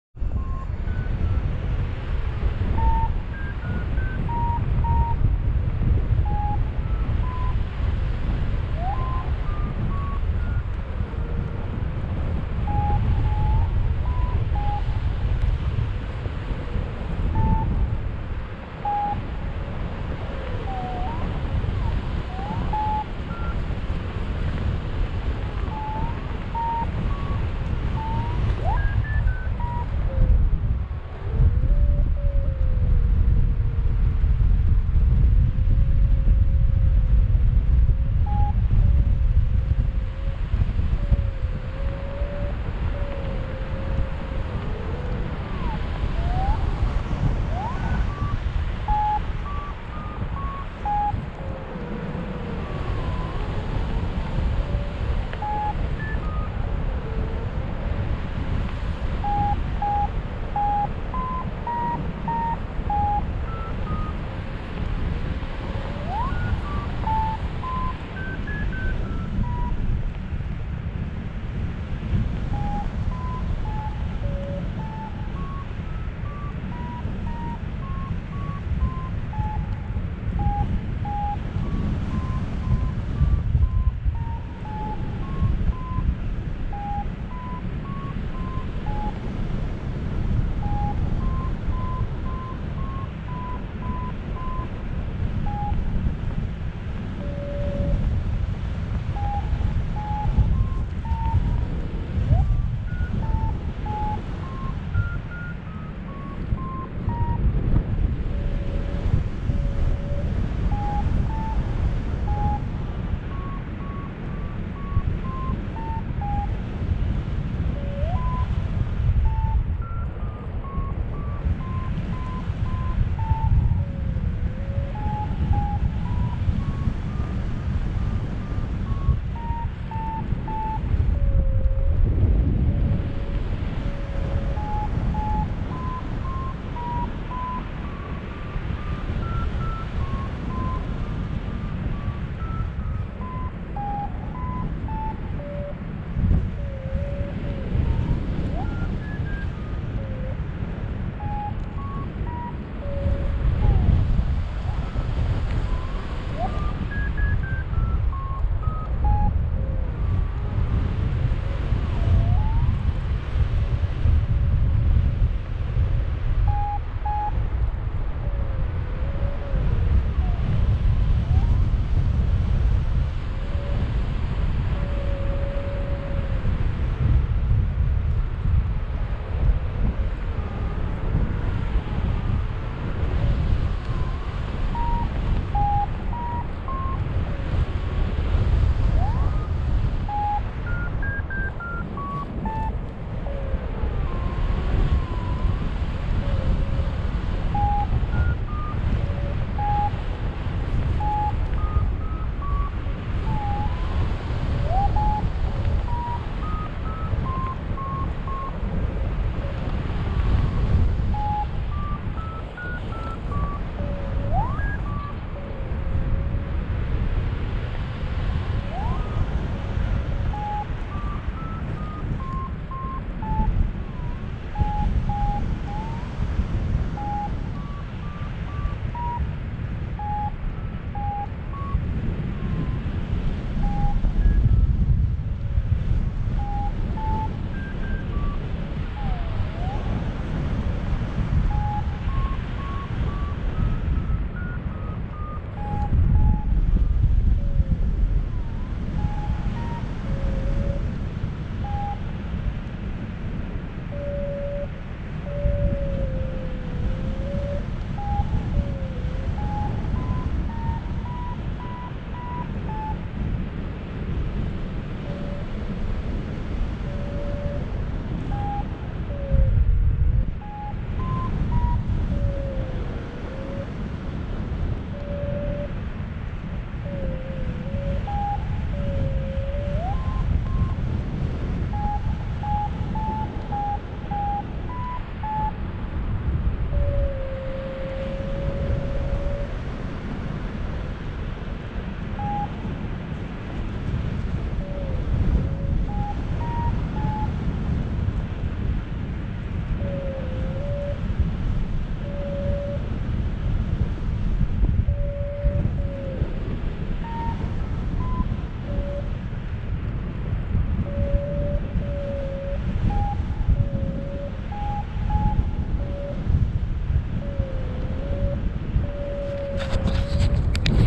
Paragling Jena - The Red Baron and soaring fun at the Thurntaler Part 3
Part three of the first flight from the Turnthaler in 2 years. That was so  much fun.